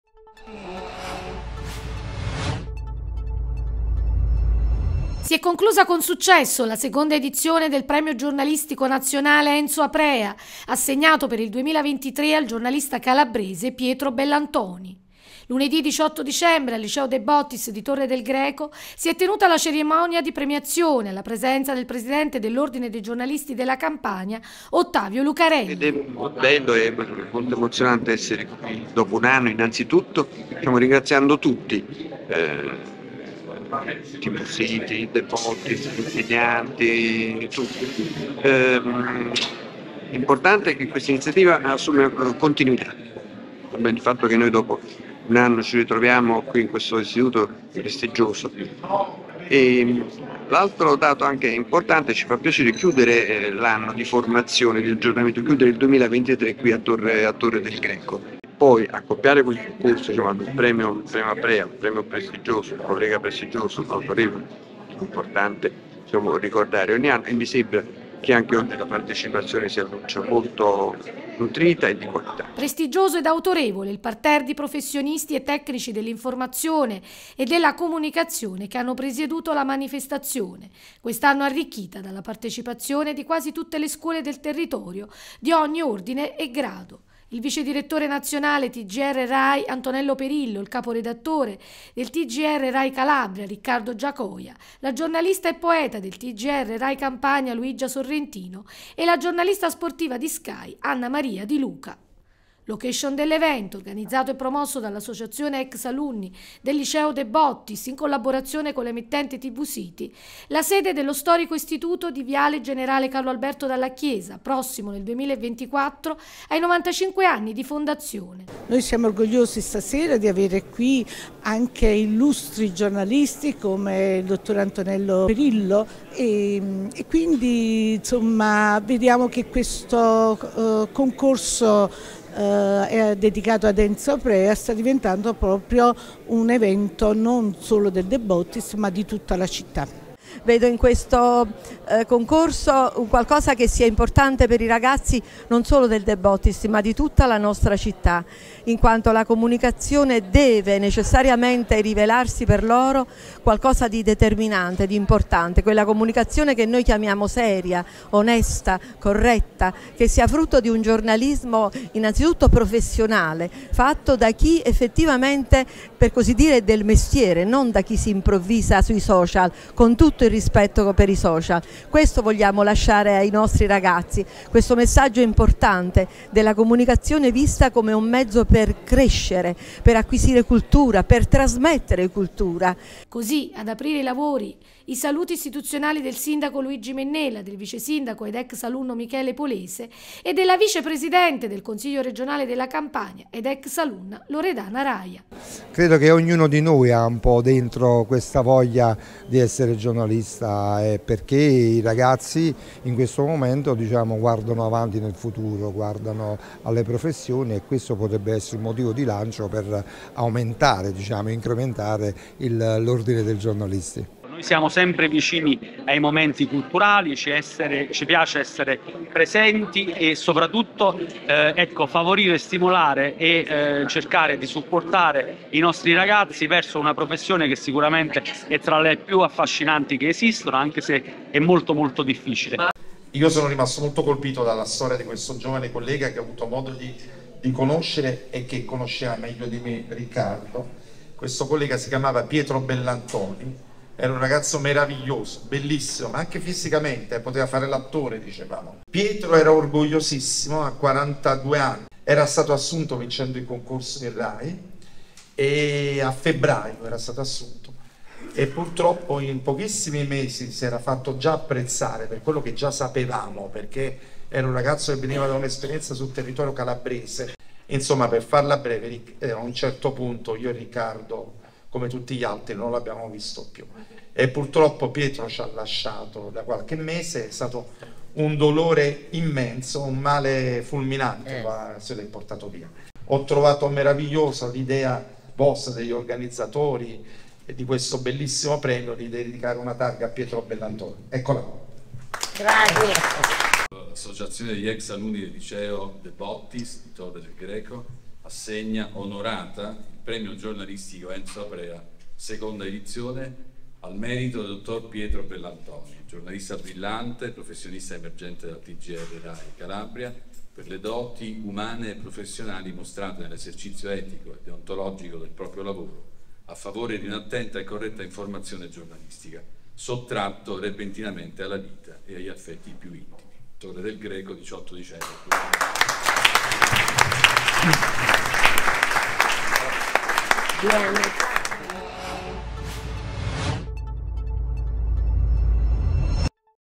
II Edizione del Premio Giornalistico "Enzo Aprea" - INTERVISTE
Si è conclusa con successo la II Edizione del Premio Giornalistico Nazionale "Enzo Aprea", assegnato per il 2023 al giornalista calabrese Pietro Bellantoni.

Lunedì 18 Dicembre al Liceo "G. de Bottis" di Torre del Greco si è tenuta la la cerimonia di premiazione alla presenza del Presidente dell'Ordine dei Giornalisti della Campania Ottavio Lucarelli.

Prestigioso ed autorevole il parterre di professionisti e tecnici dell'informazione e della comunicazione che hanno presieduto la manifestazione, quest'anno arricchita dalla partecipazione di quasi tutte le scuole del territorio di ogni ordine grado: il Vicedirettore Nazionale TGR RAI Antonello Perillo, il Caporedattore del TGR Rai Calabria, Riccardo Giacoia, la Giornalista e poeta del TGR Rai Campania, Luigia Sorrentino e la Giornalista sportiva di Sky, Anna Maria Di Luca.

Organizzato e promosso dall'Associazione "Ex alunni" del Liceo classico "G. de Bottis" in collaborazione con Tvcity presso la sede dello storico istituto di via Gen. Carlo Alberto Dalla Chiesa - prossimo nel 2024 - ai novantacinque anni di fondazione.